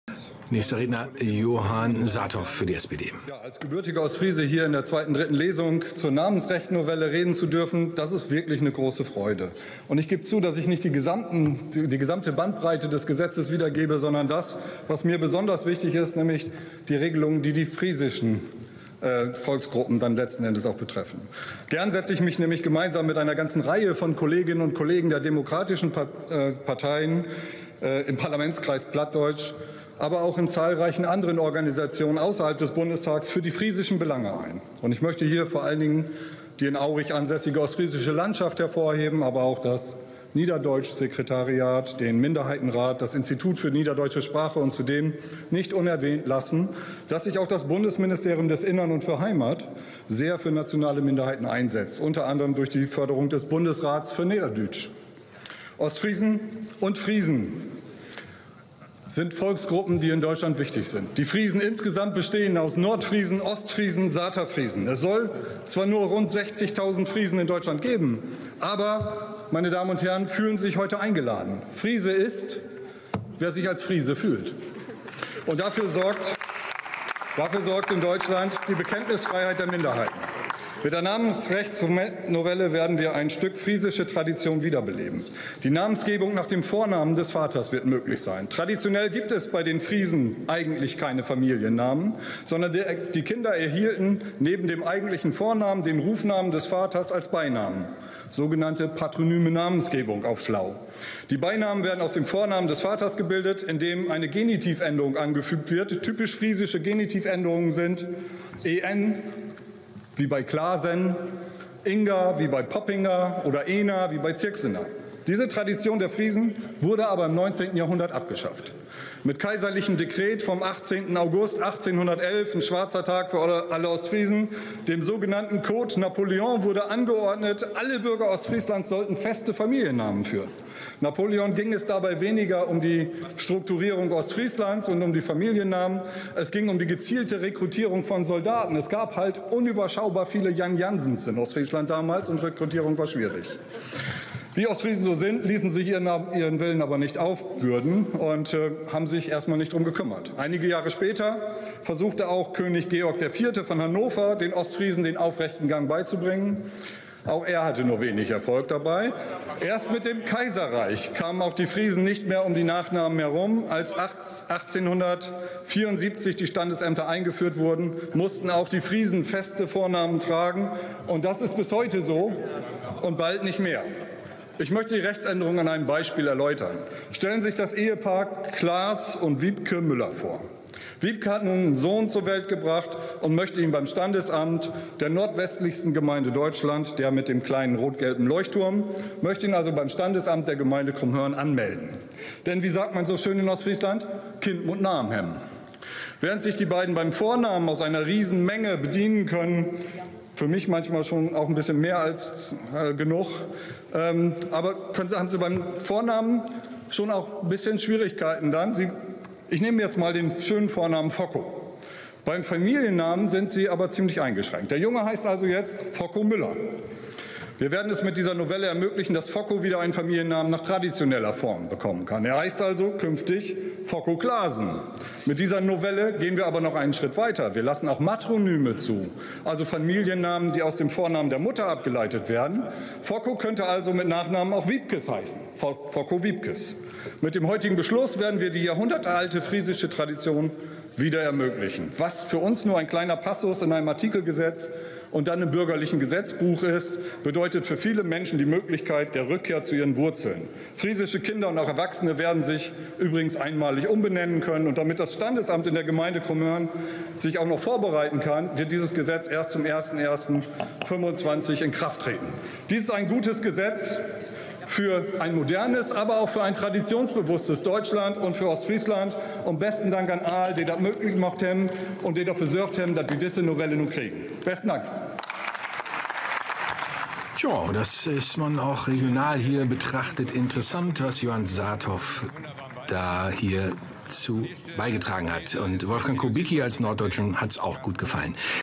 Johann Saathoff (SPD) zur Änderung des Ehenamens- und Geburtsnamensrechts am 12.04.24
Johann Saathoff (SPD) zur Änderung des Ehenamens- und Geburtsnamensrechts.